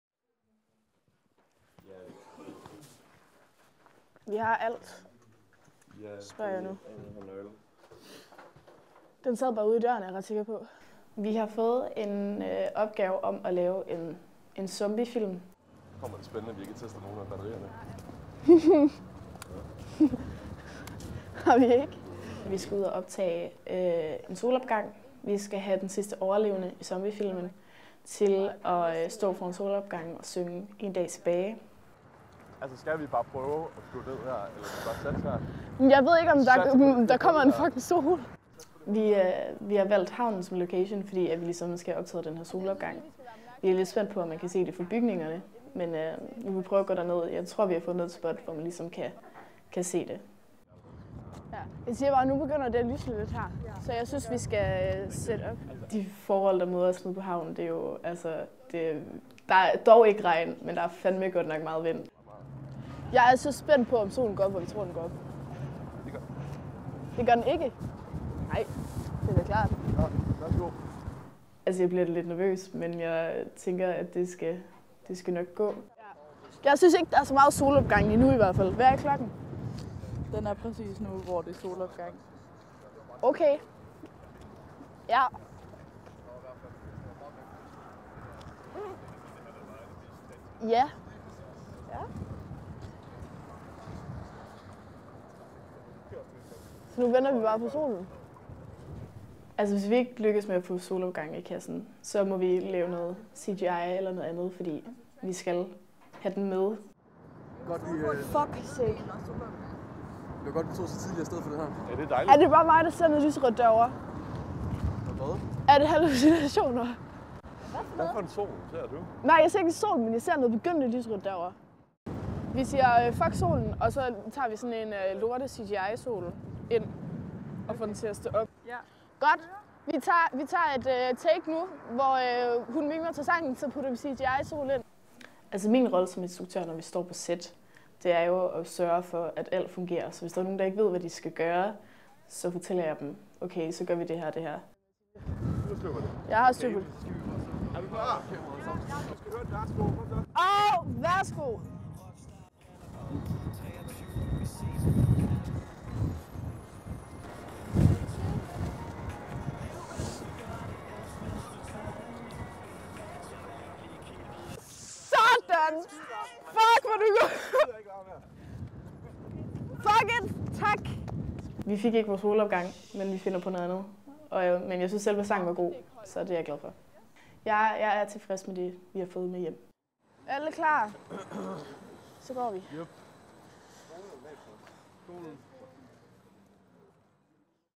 Gruppe 3: En film
En filmisk reportage om et filmhold, hvor vi følger en filminstruktør, der filmer filmstrimler til sin første film. God film.